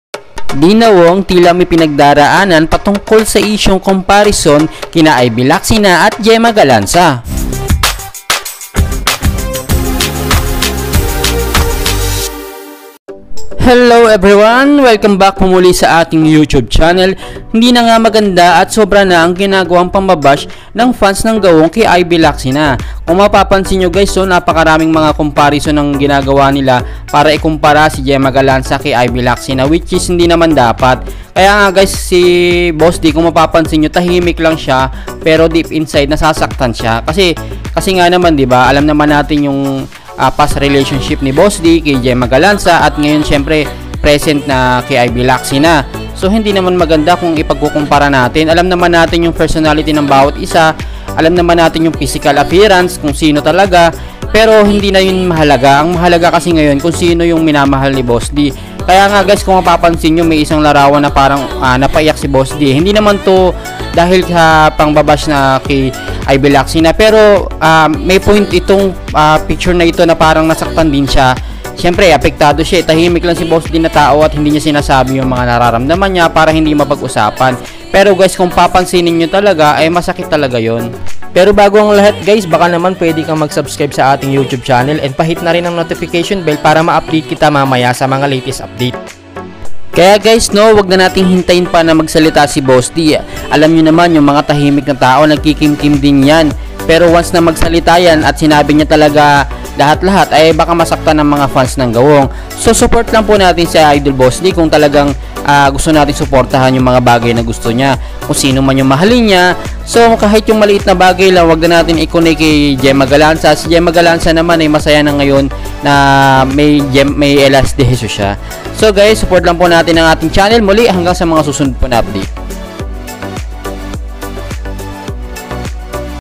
DEANNA WONG! May dinadamdam sa Mga Fans sa Pag cocompare Kay Ivy lacsina Kay Jema galanza
Tahimik si boss d pero nagdadamdam din siya sa palaging kinukumpara si Ivy lacsina Kay jema galanza.